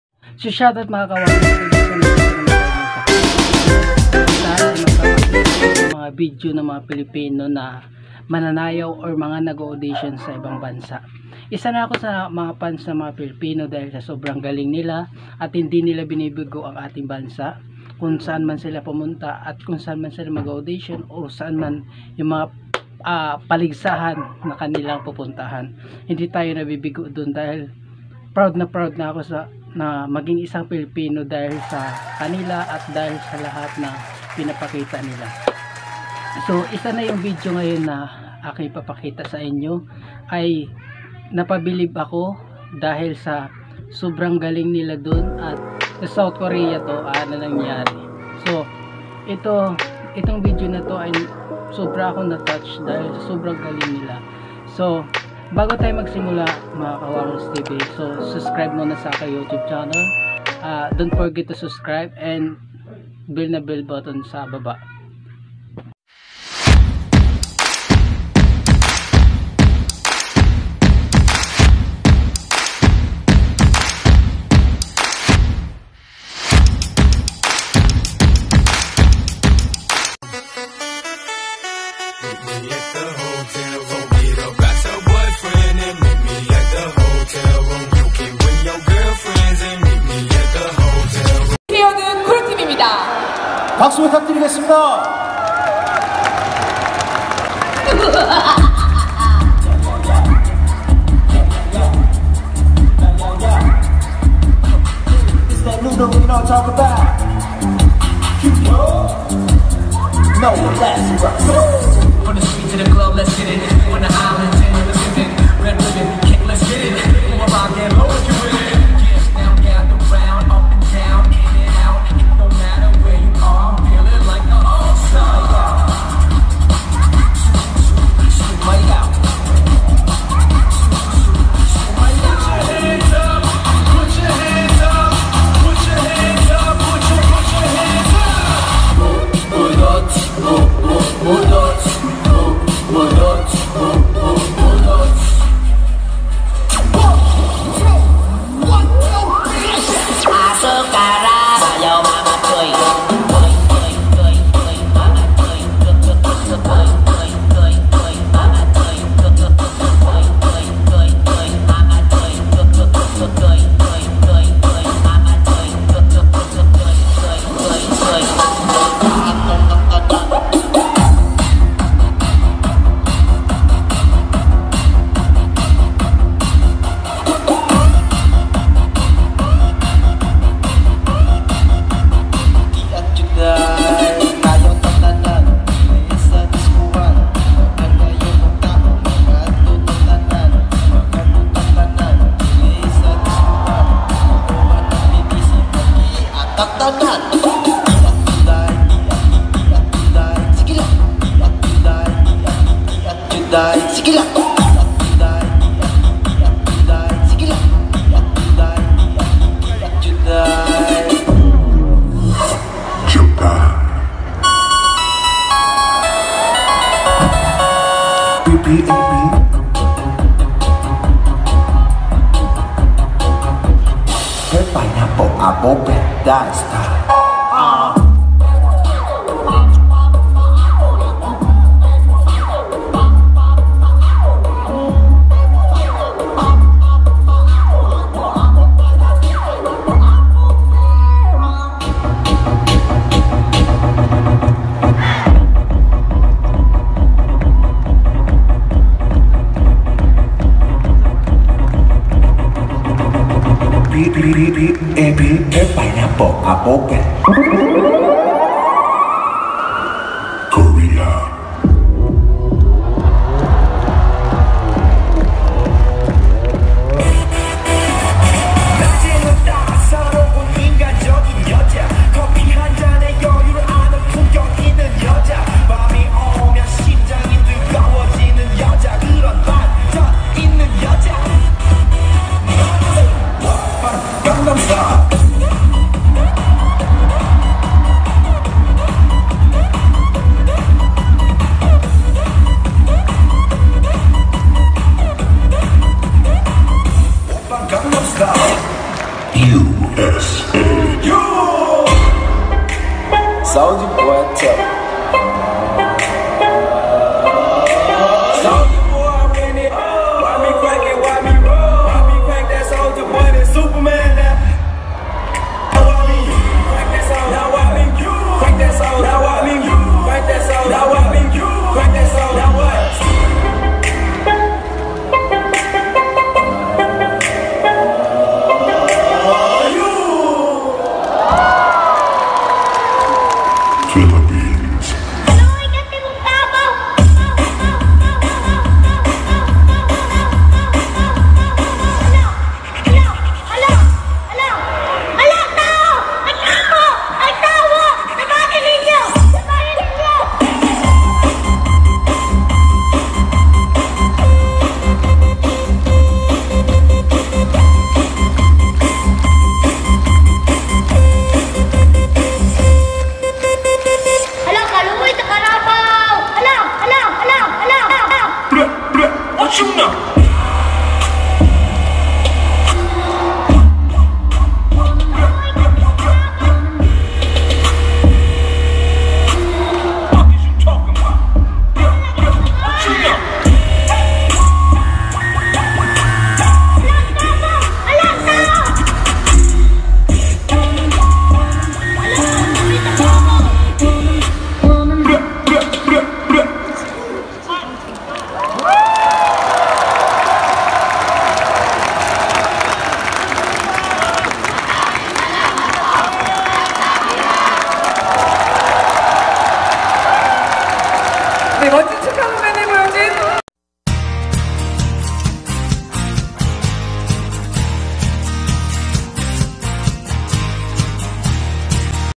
Some of the Filipino dancers showed their prowess, in South Korea| Kawanger's Tv
Main stage; 1st performance Wonju dynamic dancing carnival
Gangwon do;wonju, South Korea